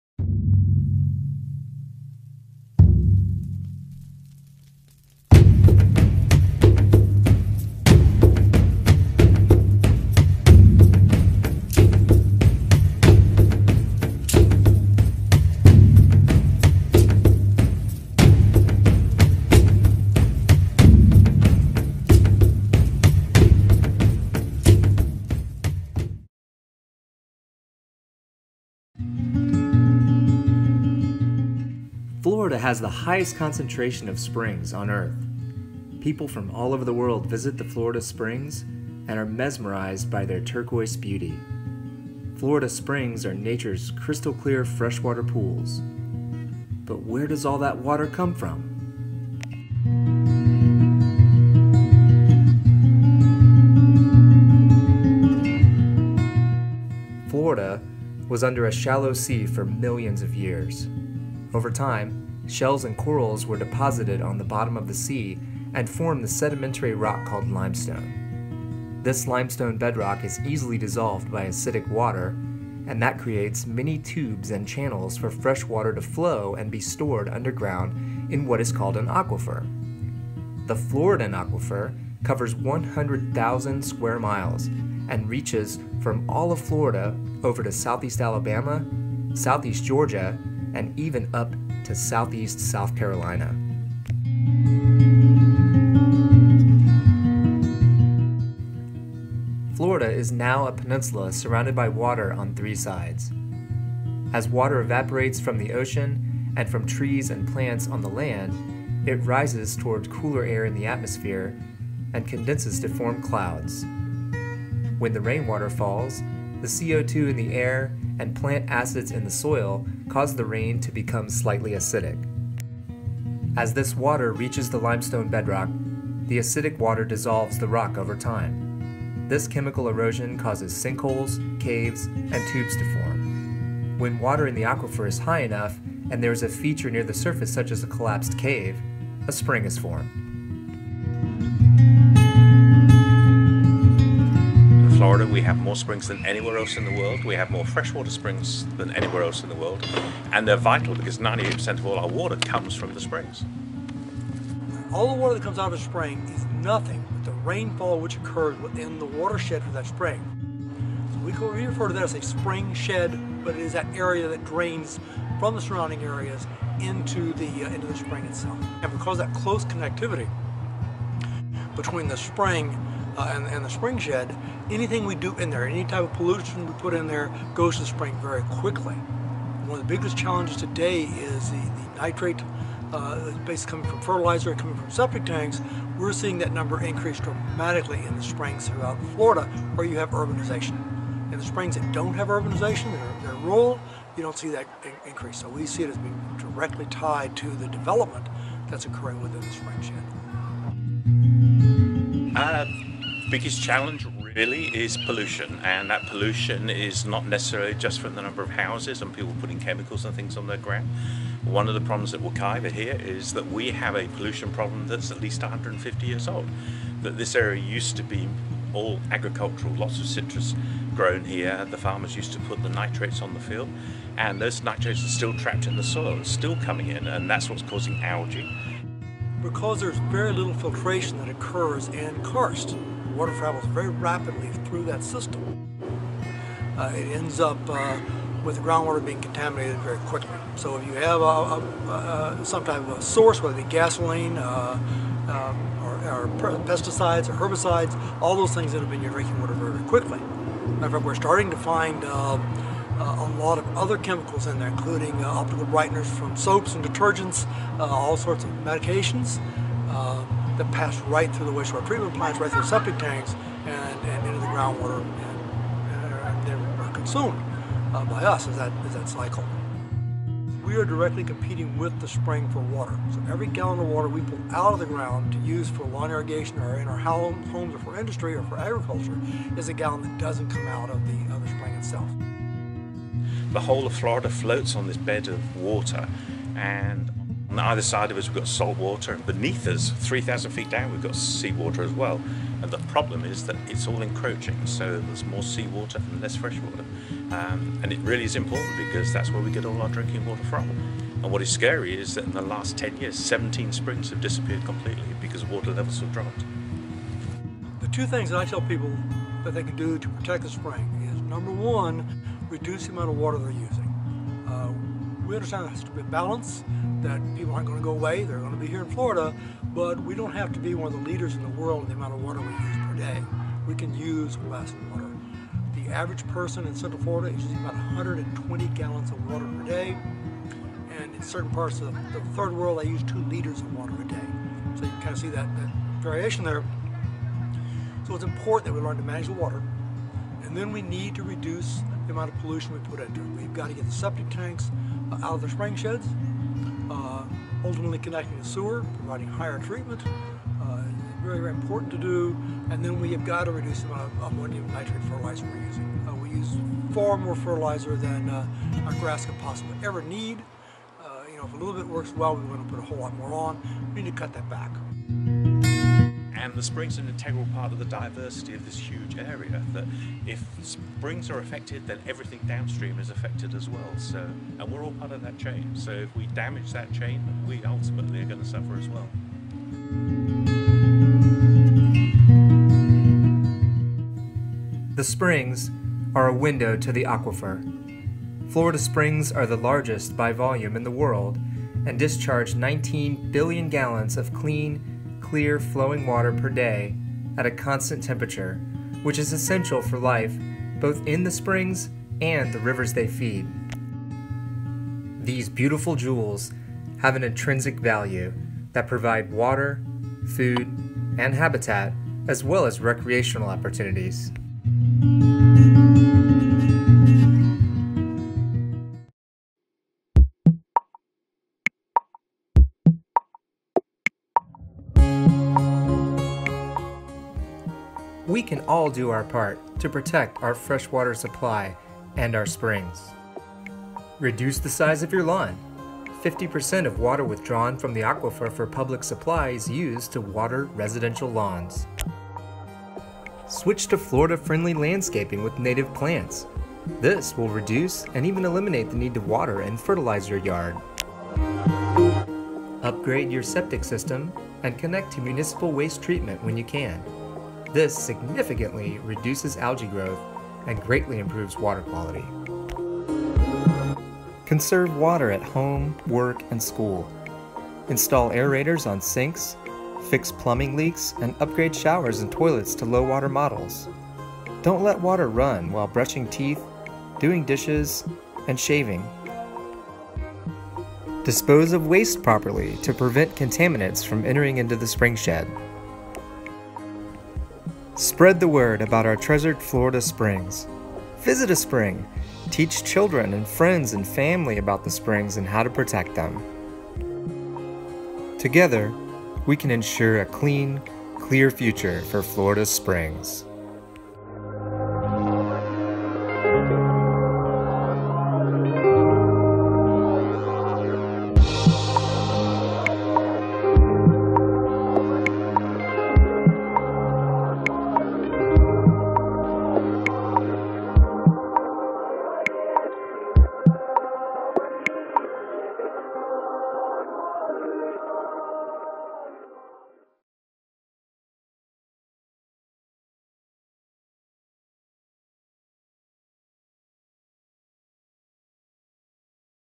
Florida Springs: Journey Through the Aquifer
Florida has a greater concentration of fresh water springs than anywhere else in the world. Join us as we discover where this crystal clear gift from the earth comes from.